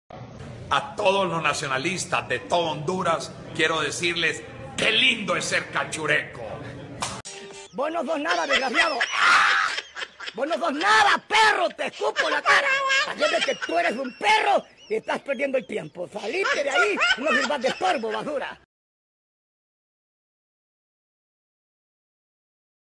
chago le escupe la cara a Tito asfura.
Video de EL CHARRULA REVOLUCIONARIO.